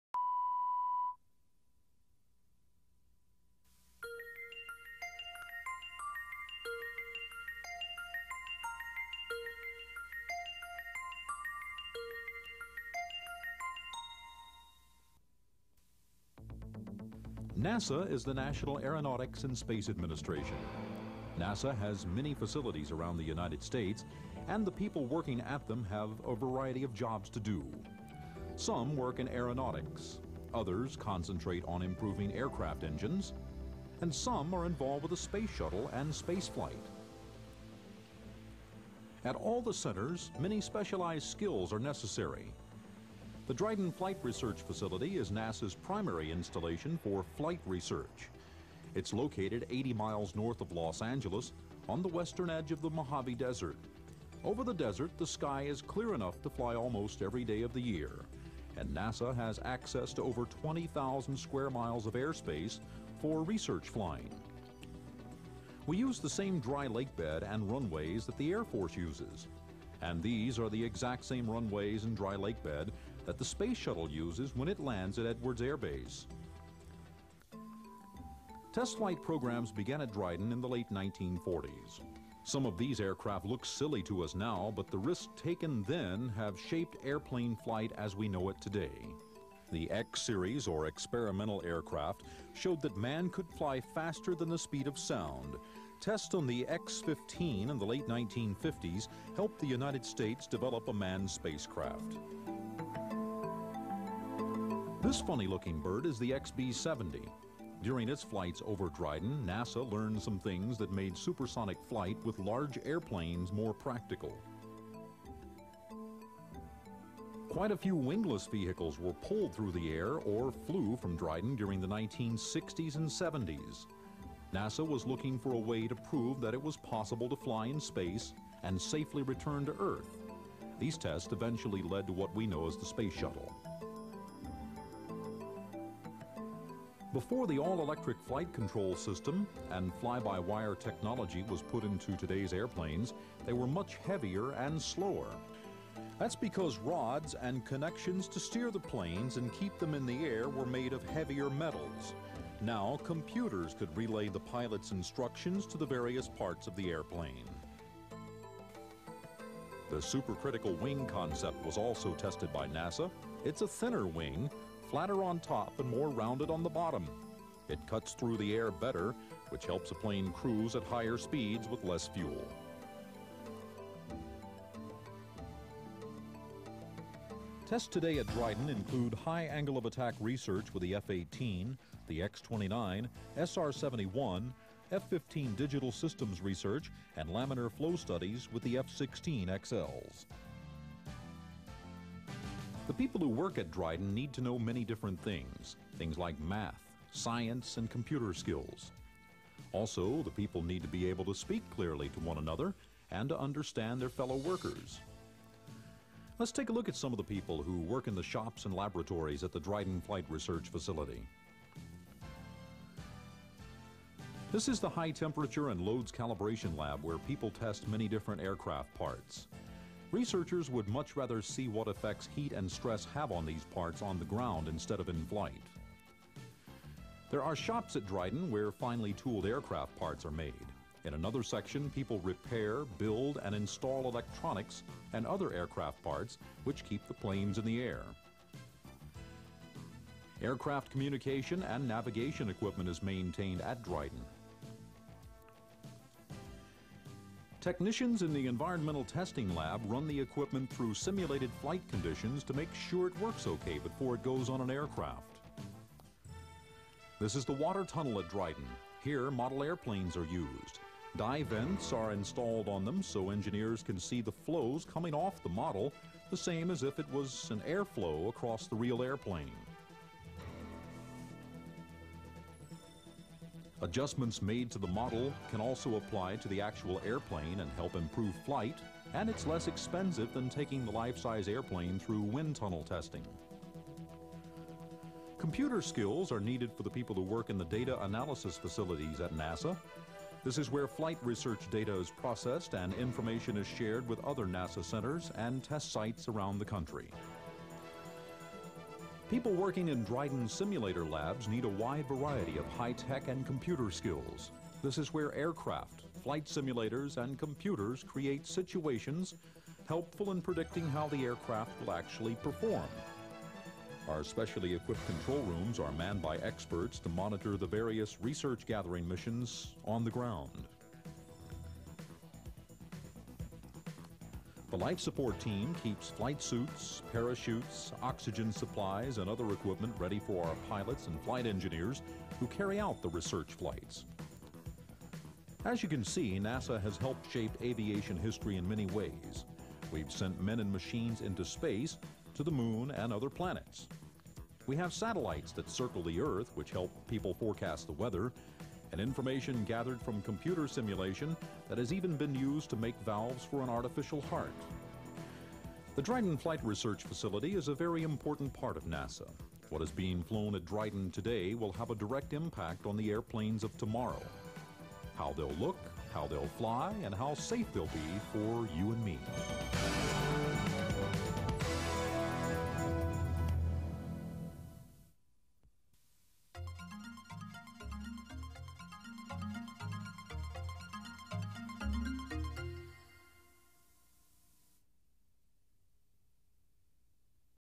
Dryden Overview for Schools
This video provides educators an overview of Dryden for students from late elementary through high school.